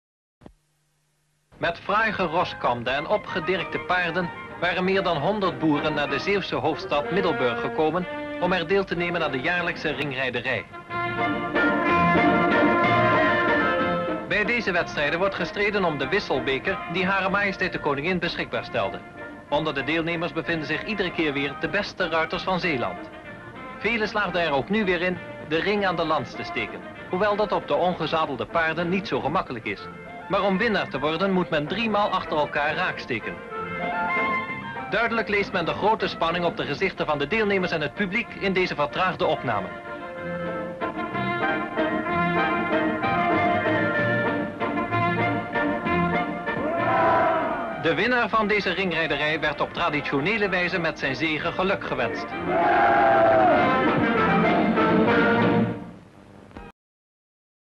Tilt at the ring in the capital of Zeeland (Week number 52-35) 🔴
Description (more): Famer's sons from Zeeland compete against eachother on unsaddled horses in the annual tilt at the ring in Middelburg. Audience and participants are mostly in traditional Zeeland attire. By the old tradition, the winner is tossed in the air.